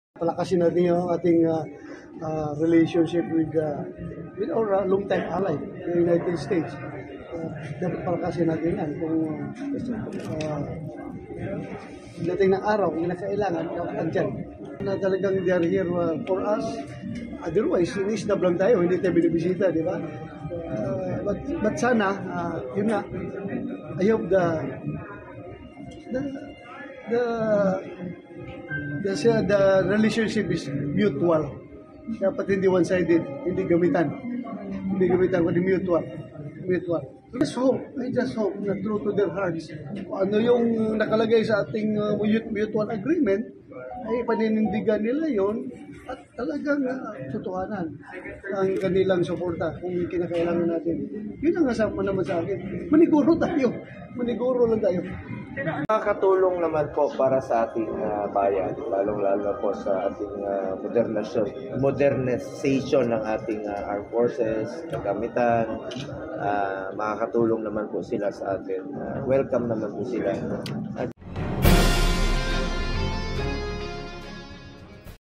Pagbisita ni VP Harris makatutulong sa pagpapalakas ng alyansa sa US
Naniniwala si Senador Ronald 'Bato' dela Rosa na ang pagbisita sa bansa ni United States Vice President Kamala Harris ay pagkakataon para mapalakas pa ang alyansa ng Pilipinas sa Estados Unidos.

Ayon kay Dela Rosa, dapat lang na pagtibayin ang relasyon natin sa US para sakaling kailanganin natin ng tulong ay matitiyak na natin ang suporta mula sa dambuhalang bansa.

Kasabay nito, umaasa ang senador na mapapanindigan talaga ang mga nakasaad sa Philippines-US mutual defense treaty.

Welcome rin para kay Senador Christopher 'Bong' Go ang pagbisita ng US Vice President sa bansa.

Lalo na aniya kung makakatulong ito sa ating mga kababayan at sa modernisasyon ng mga kagamitan ng ating sandatahang lakas.

Umaasa rin si Go na matatalakay sa pagpupulong  nila Pangulong Ferdinand R. Marcos Jr. at VP Harris ang mga usaping makakatulong sa ating ekonomiya, peace and order at sa isyu sa West Philippine Sea.